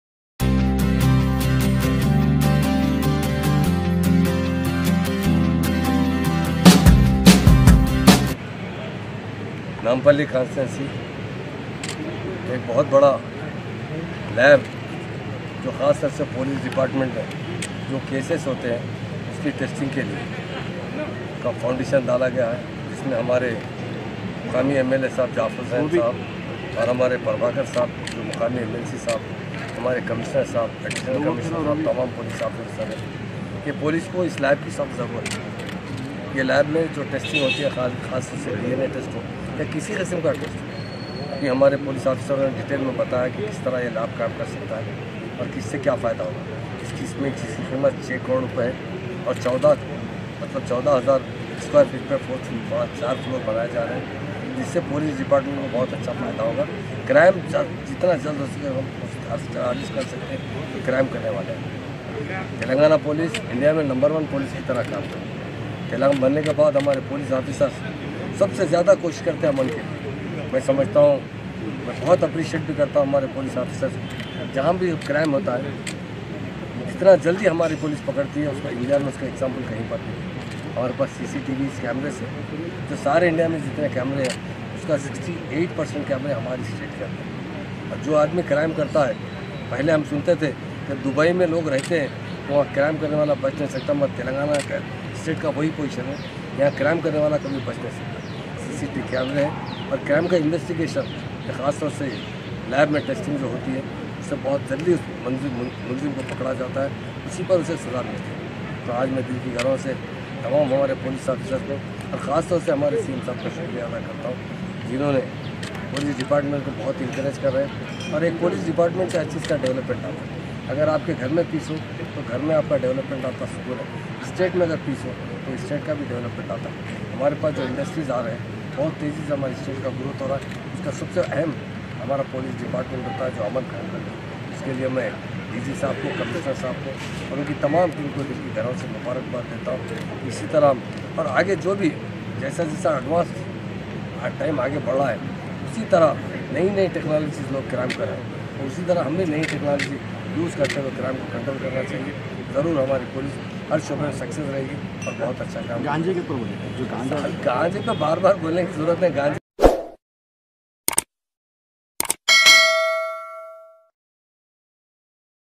DNA మాలిక్యులర్ బయాలజీ ల్యాబ్ కు శంకుస్థాపన చేసిన హోం శాఖ మంత్రి మహమూద్ అలీTelangana times news
DNA Molecular biology lab Inaugurated by Home Minster Mehmood Ali garu